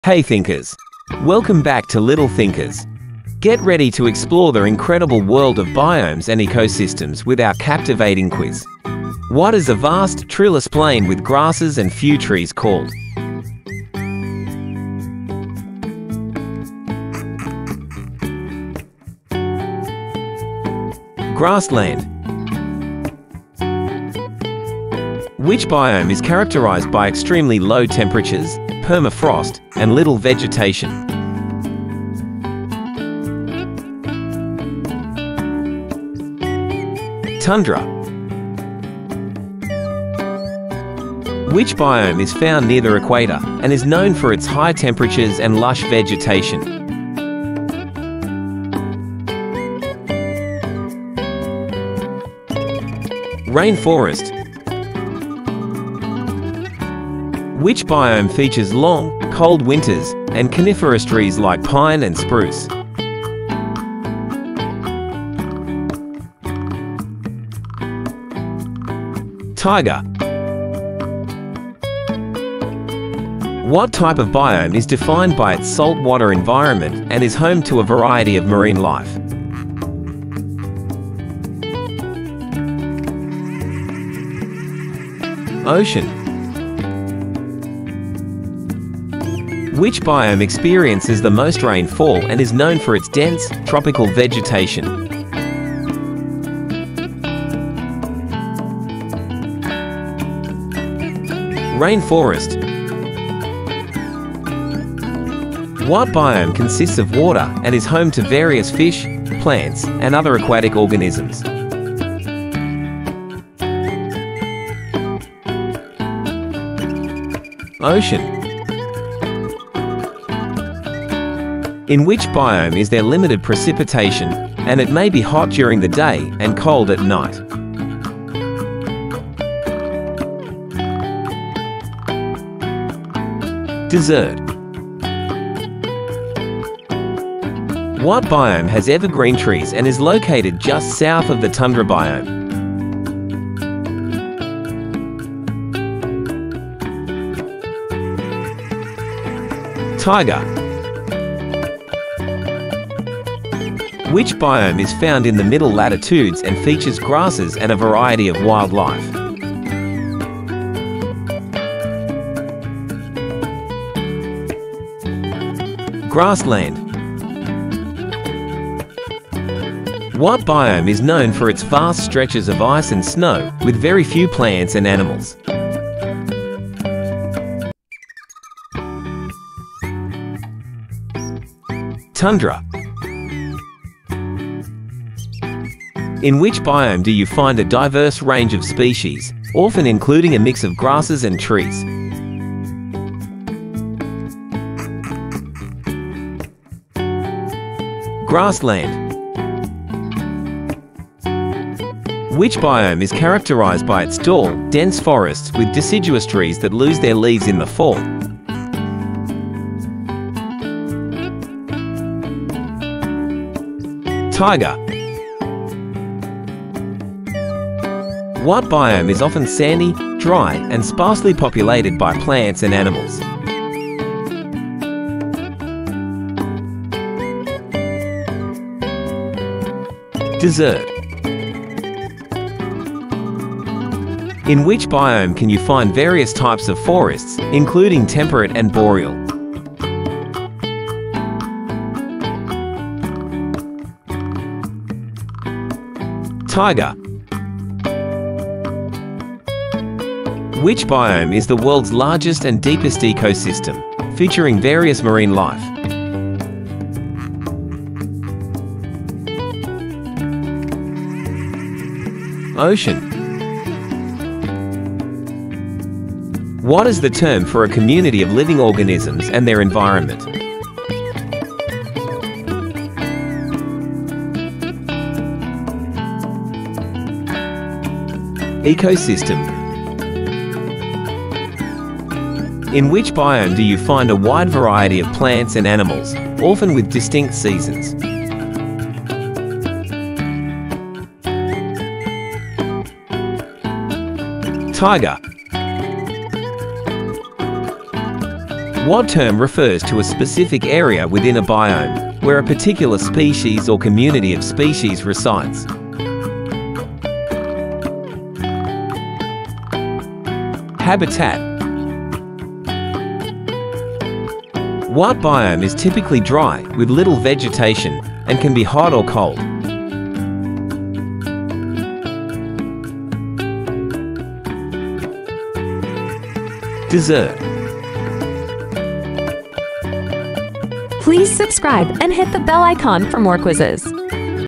Geography Quiz - Biomes and Ecosystems for kids | Little Thinkers
Geography Quiz - Biomes and Ecosystems for kids

"Welcome to an awe-inspiring expedition through the tapestry of life on Earth! In this immersive video, we embark on an exhilarating exploration of Biomes and Ecosystems, unravelling the intricate harmony between environments and the living beings within them.

Prepare to be mesmerized as we traverse the planet's diverse landscapes, from the enchanting depths of vibrant rainforests teeming with life to the stark, serene beauty of sprawling deserts. Join us on this educational odyssey as we delve into the fascinating intricacies of each biome, shedding light on the extraordinary adaptations and coexistence among its inhabitants.

Discover the delicate dance of nature's balance, where every organism plays a crucial role in maintaining the ecosystem's health. From the microscopic world of soil microbes to the majestic presence of apex predators, witness the interconnectedness that sustains life in these dynamic habitats.

But it's not just about exploration; it's a call to action. Through this visual voyage, we highlight the urgent need to conserve and protect these invaluable ecosystems. Learn about the threats these biomes face, from climate change to human impact, and join us in the collective effort to preserve these natural wonders for generations to come.

Whether you're a nature enthusiast, a student eager to uncover the secrets of the natural world, or simply someone captivated by the beauty of our planet, this video promises to captivate and educate.

Come, be a part of this enthralling journey into the heart of Biomes and Ecosystems, where every frame is a celebration of Earth's astounding biodiversity and a testament to the interconnected web of life. Join us in this passionate tribute to our planet's irreplaceable wonders! 🌍🌿✨ Biomes Ecosystems NatureEducation Conservation"